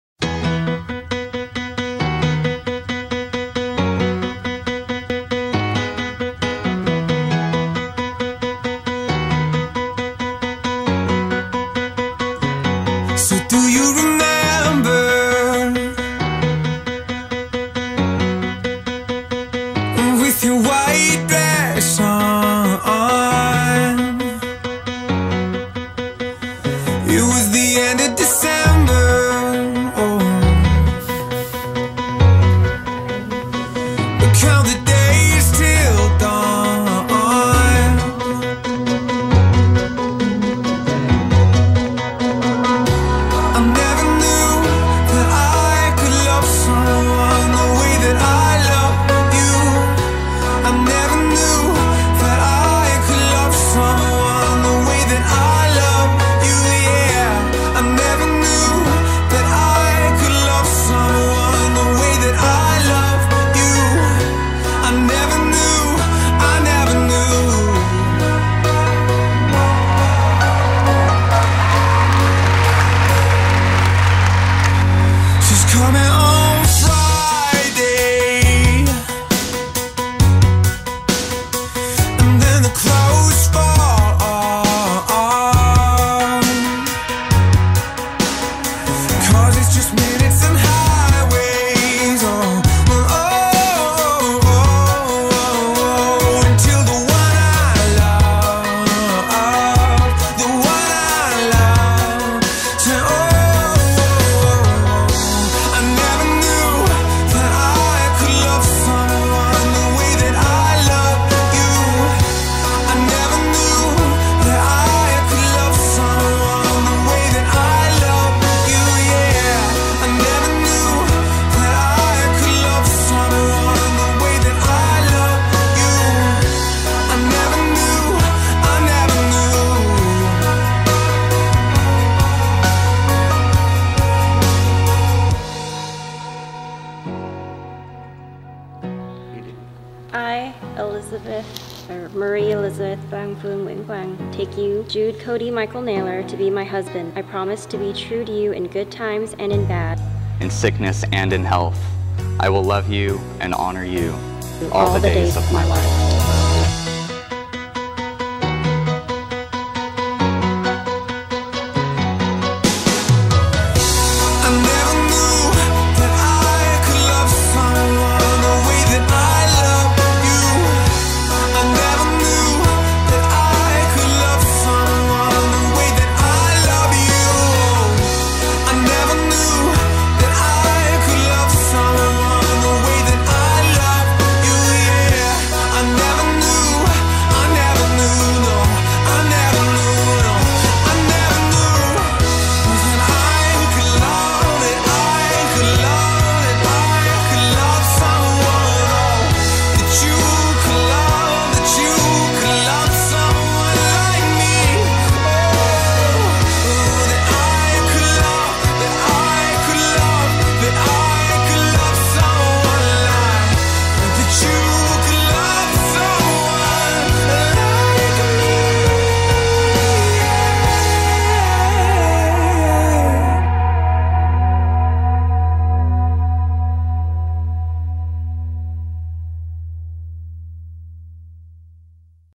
| Wedding | SF Bay Area Wedding - Highlight [Liz & Cody]
Watch in HD!! A SF Bay Area Wedding Video.

Ceremony:  Church of the Resurrection, Sunnyvale
Reception: Joy Luck Palace, Cupertino